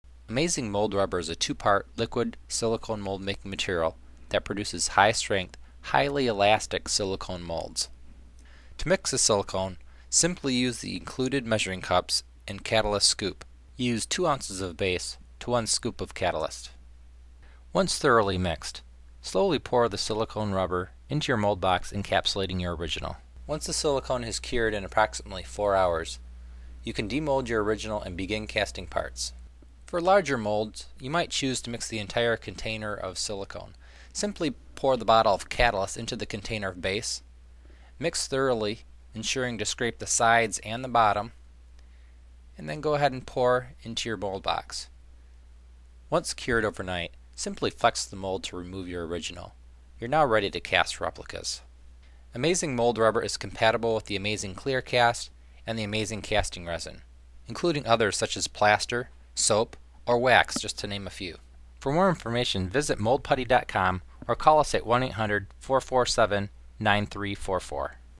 Amazing Mold Rubber
Amazing Mold Rubber is a high strength, highly elastic liquid silicone mold making material.  It's used to make rubber molds that reproduce exact replicas of originals right down to the tiniest of details.  It's easy to use mix ratio and thin consistency makes degassing unnecessary. for additional product information and video tutorials.